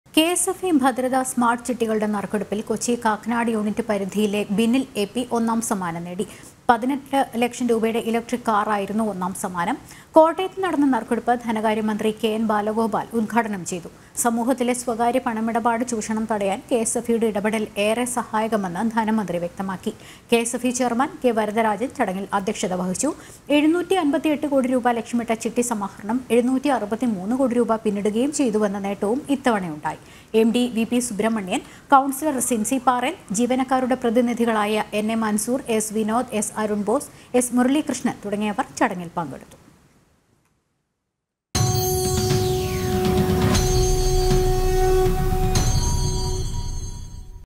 KSFE ഭദ്രതാ സ്മാർട്ട് ചിട്ടികളുടെ നറുക്കെടുപ്പിൽ ബിനിൽ എ പി ഒന്നാം സമ്മാനം നേടി
ksfe ഭദ്രതാ സ്മാർട്ട് ചിട്ടികളുടെ നറുക്കെടുപ്പിൽ കൊച്ചി കാക്കനാട് യൂണിറ്റ് പരിധിയിലെ ബിനിൽ എ പി ഒന്നാം സമ്മാനം നേടി